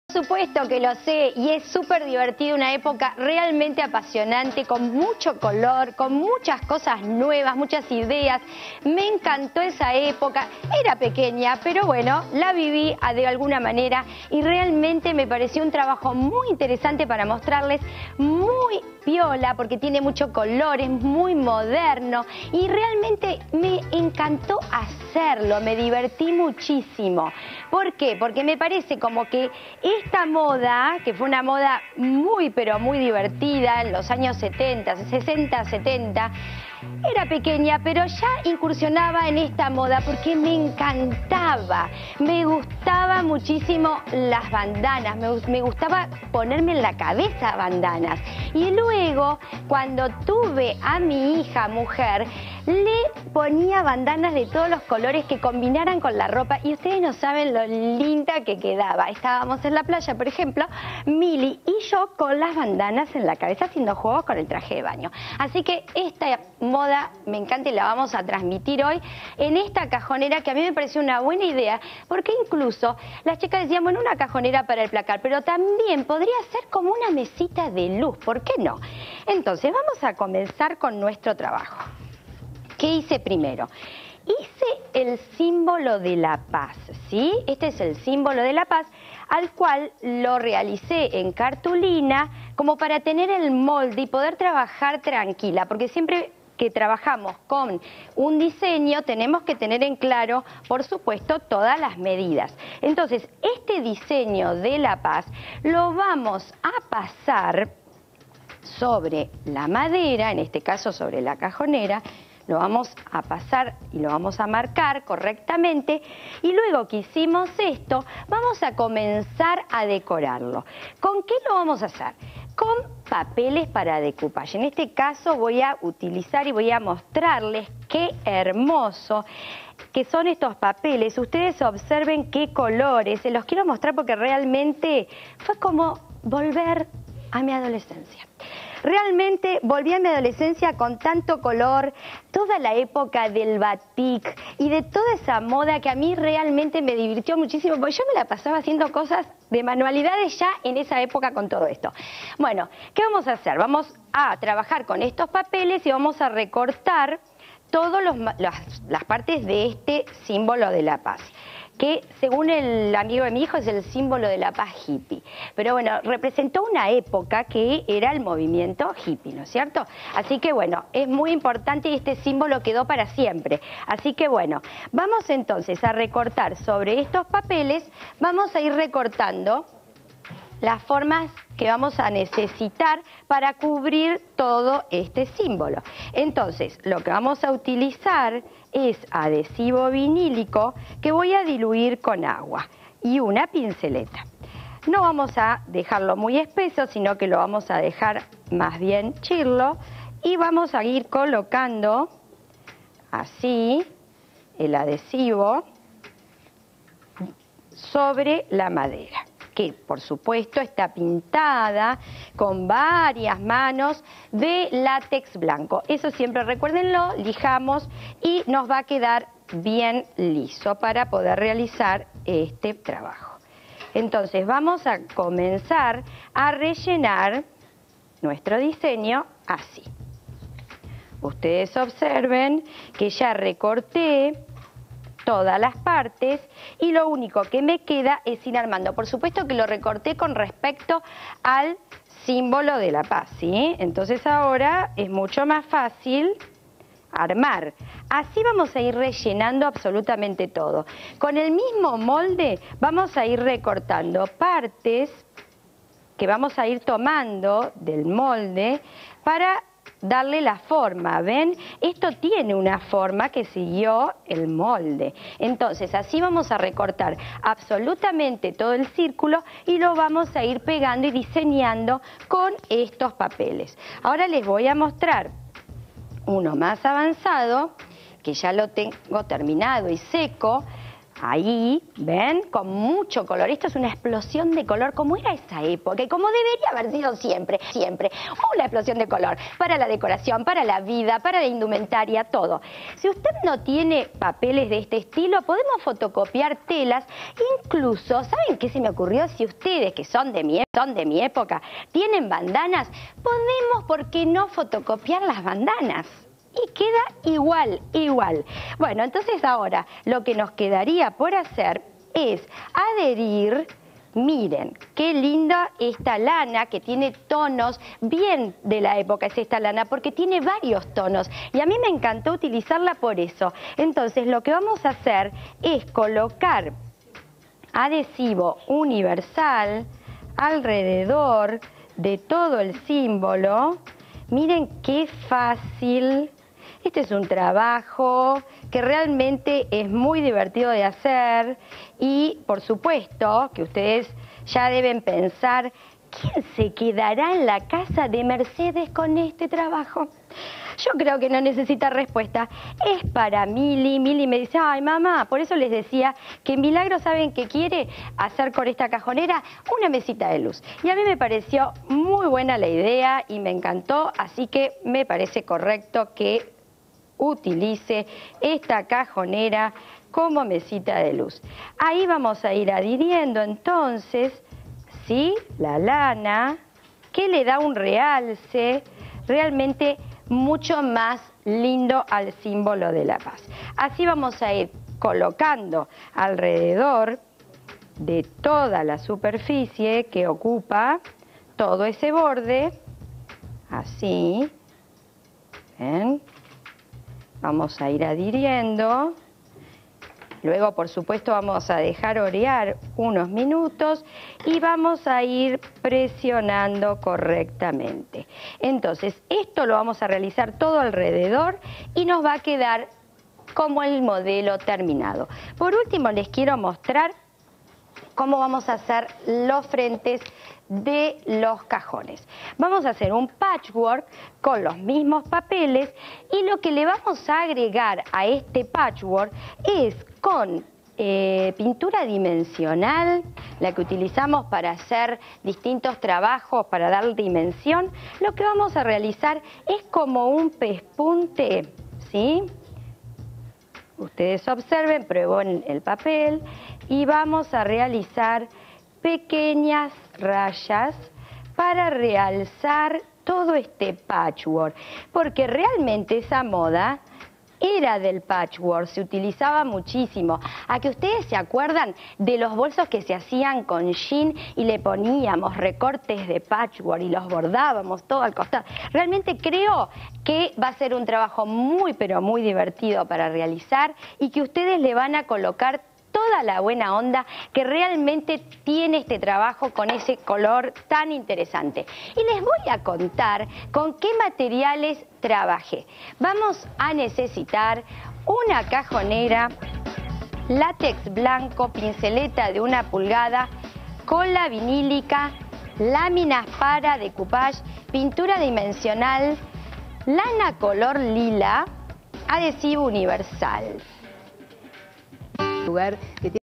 Desde casa - cajonera con papeles
Mercedes Gutierrez hace un lindo cajonera con papeles, no tengo derecho sobre el  vídeo